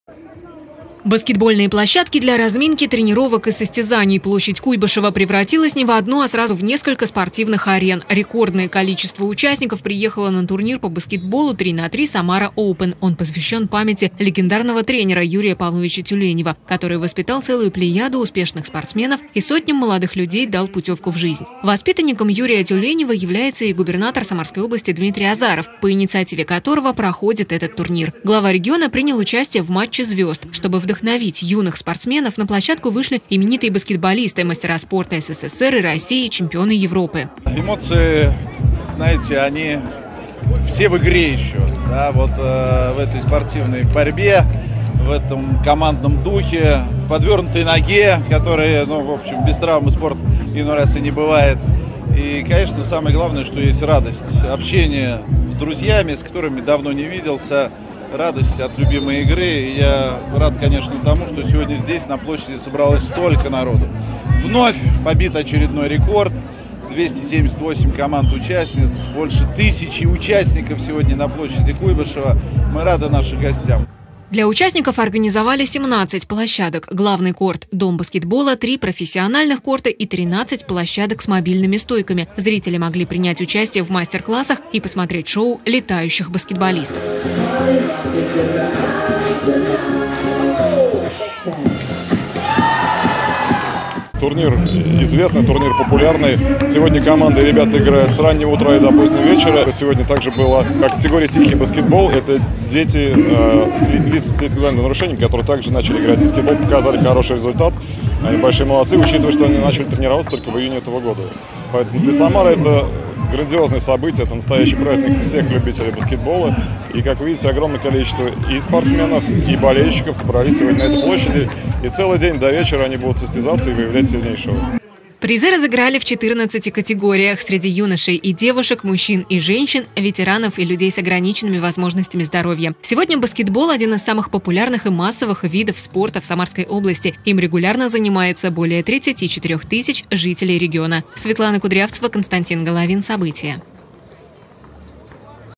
Состоялся традиционный турнир по баскетболу 3*3 Samara Open
В Самаре состоялся тринадцатый турнир по баскетболу три на три "Самара Оупэн" памяти заслуженного тренера России Юрия Павловича Тюленева. В этом году показать свое мастерство приехало рекордное количество спортсменов - 278 городов из разных регионов. С подробностями - Светлана Кудрявцева.